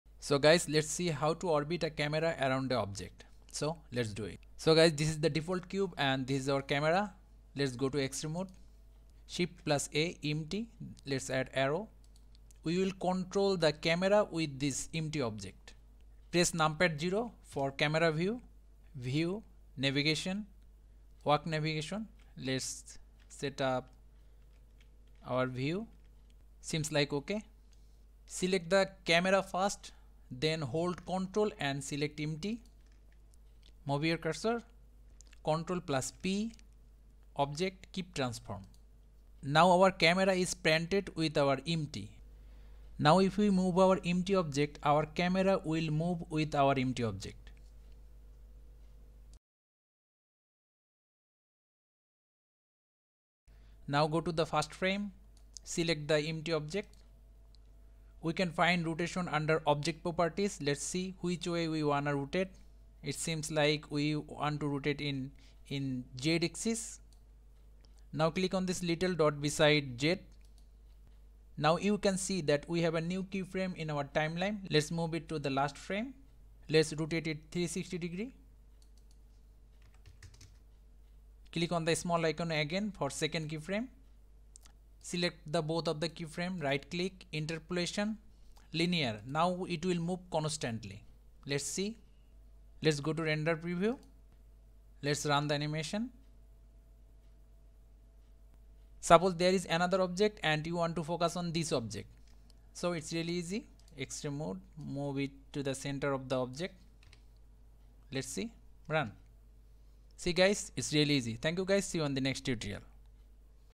Blender Tutorial : How to Rotate or Orbit Camera Around Object in Blender 3.1
How to Circle , rotate or orbit camera around any object in blender 3.1 . How to easily shift camera focus on different object.